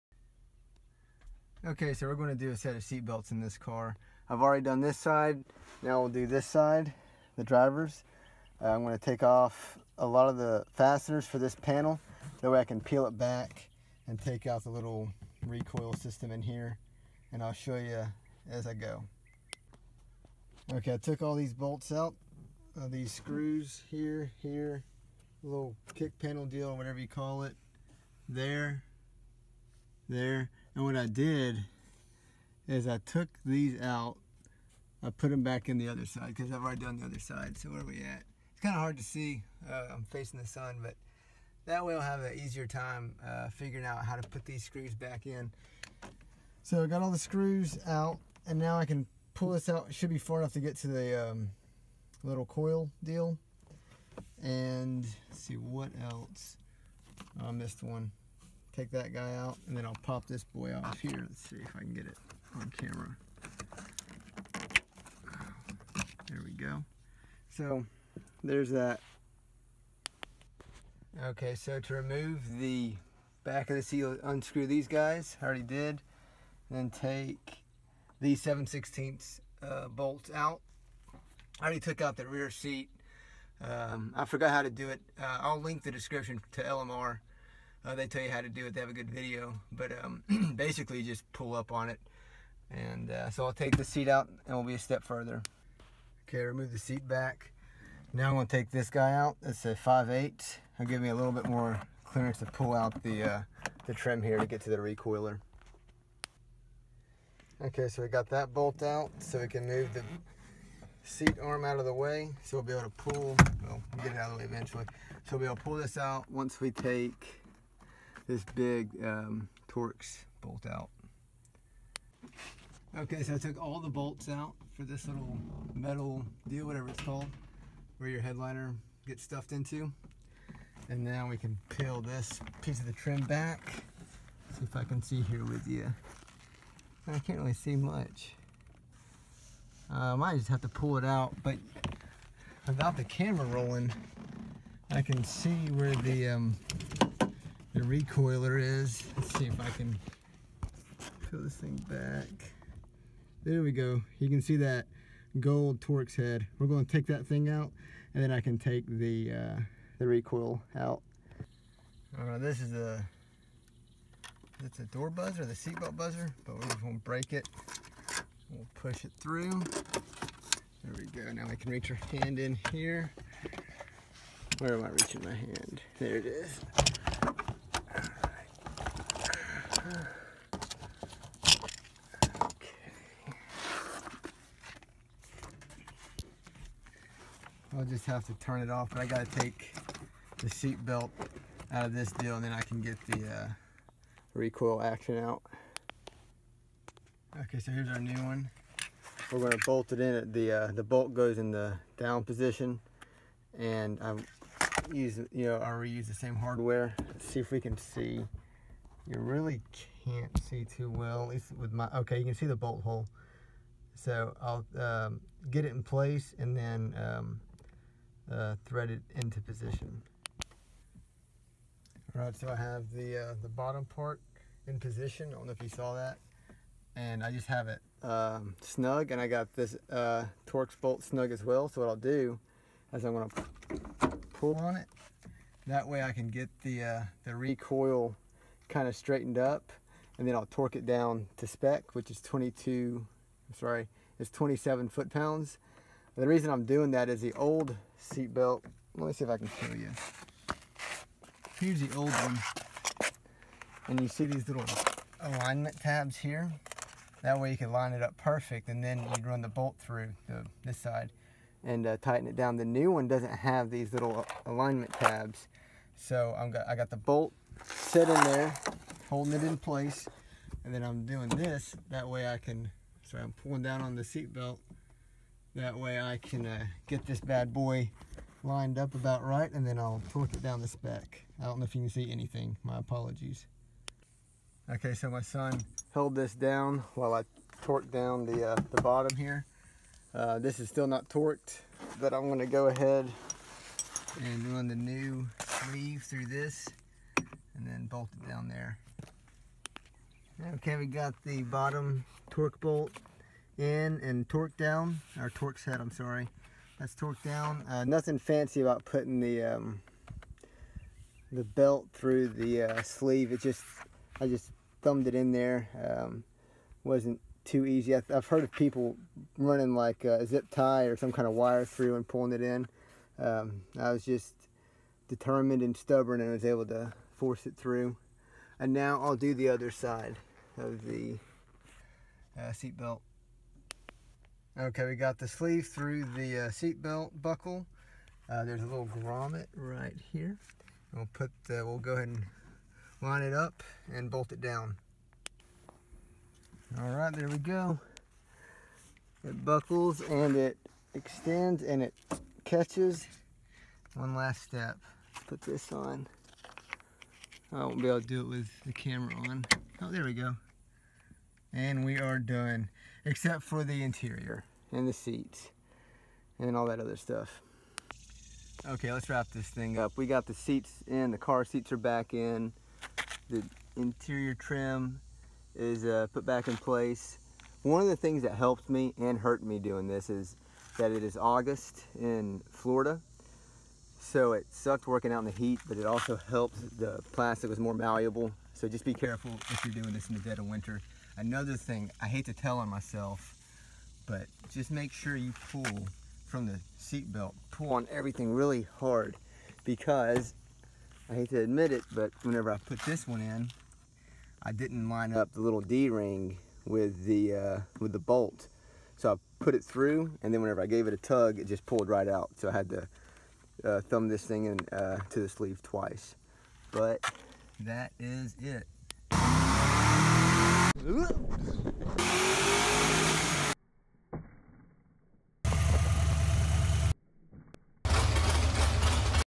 How to change seatbelts in 1979-89 Mustang / Ford Vehicle
Changing seatbelts in a 1984 Mustang using LMR's front seat belt kit part no:  LRS-61109EK
If you forgot how to remove the back seat (like I did) here's a sweet vid: